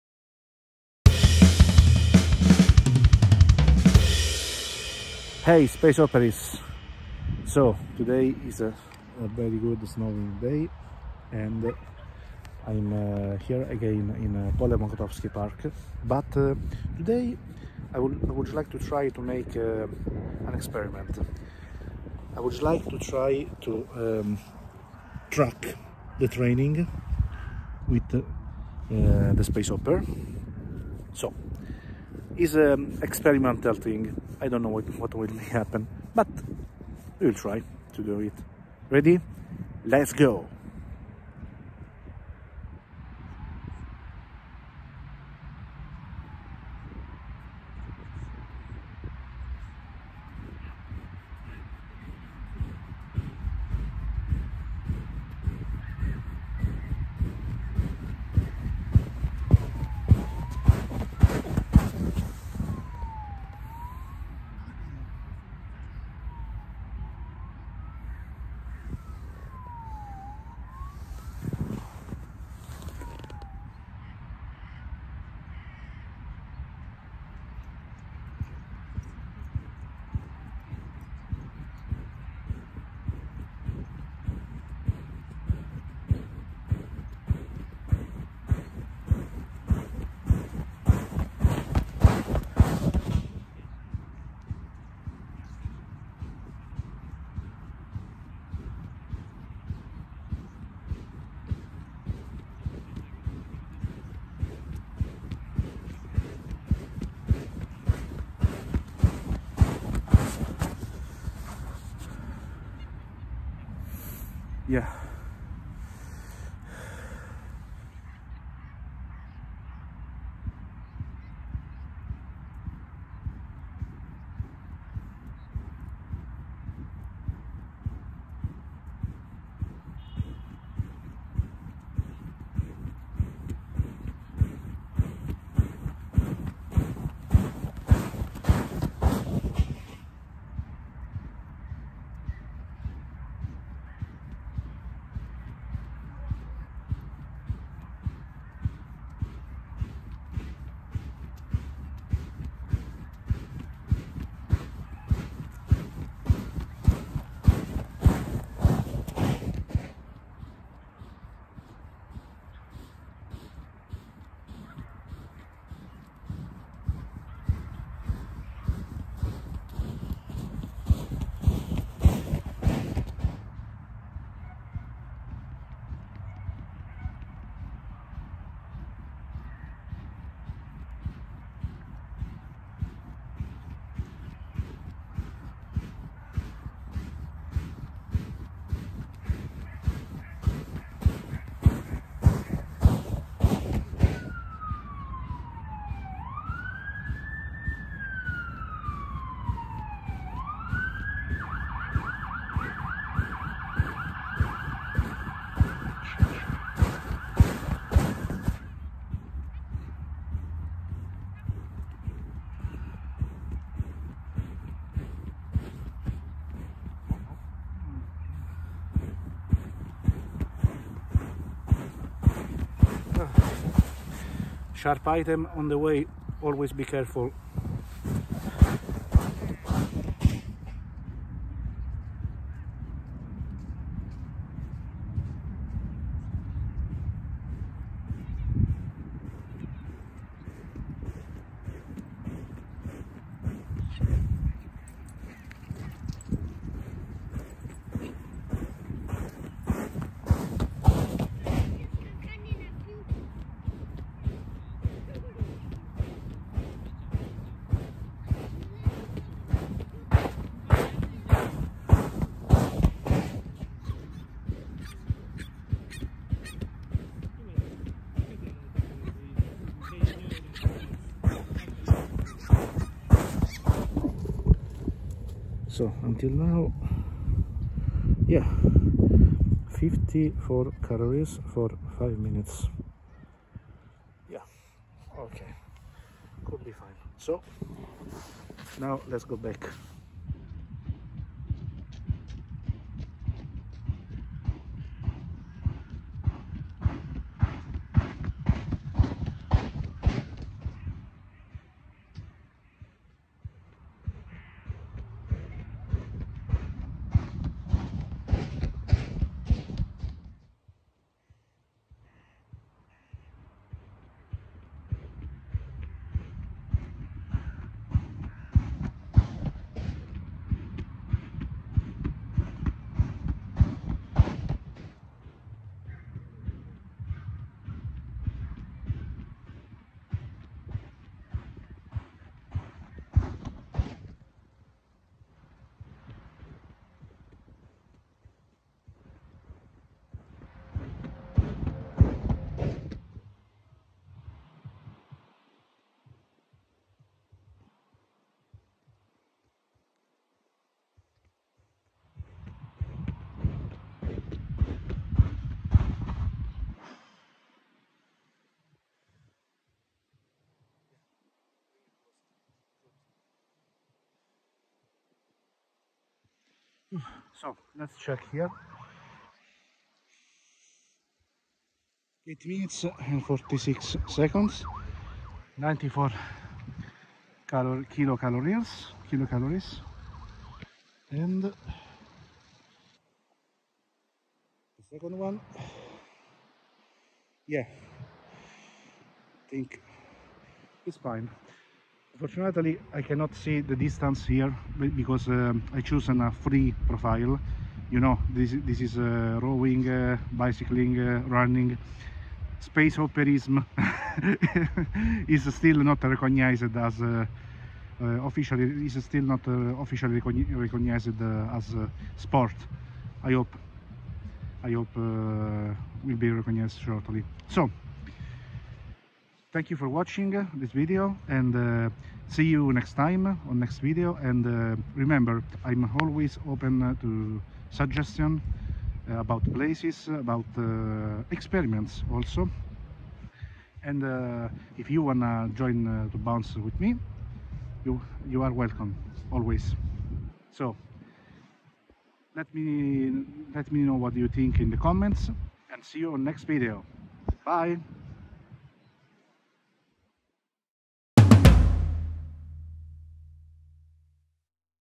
(Experimental) Tracking Space Hopper bounce training with Smartwatch
Tracking space hopper training with smartwatch...is possible?